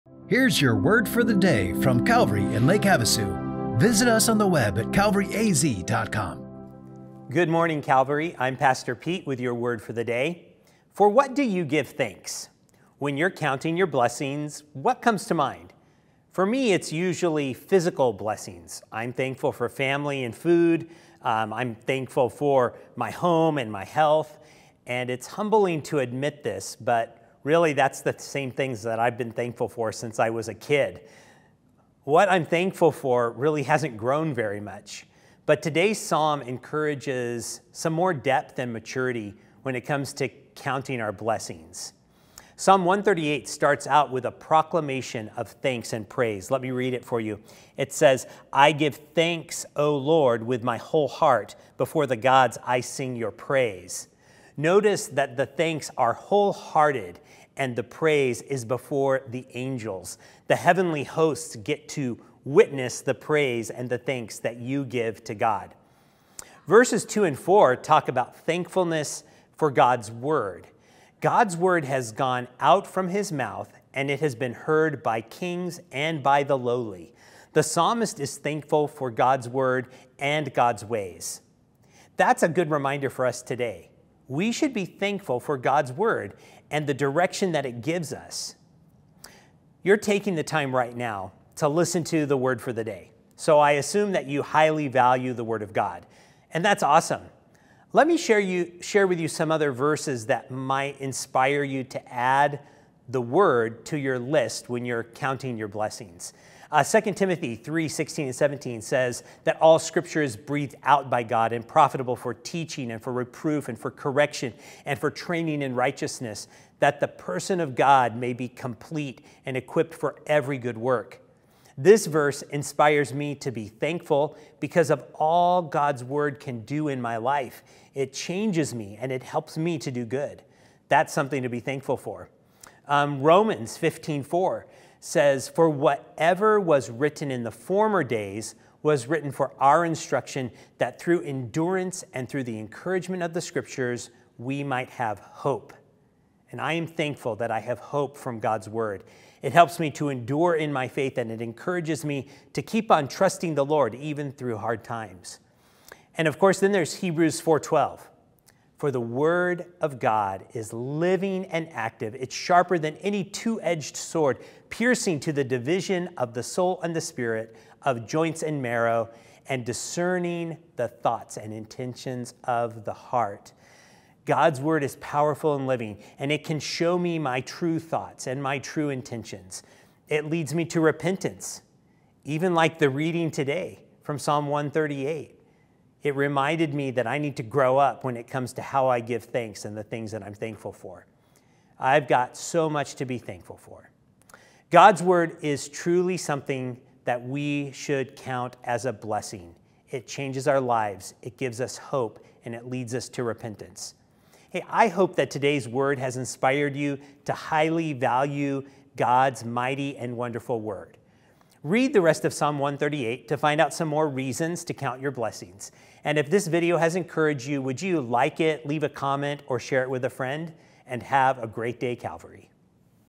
Your Word for the Day - Episode 867
Pastor Pete acknowledges that we have much more to be thankful for than we typically believe.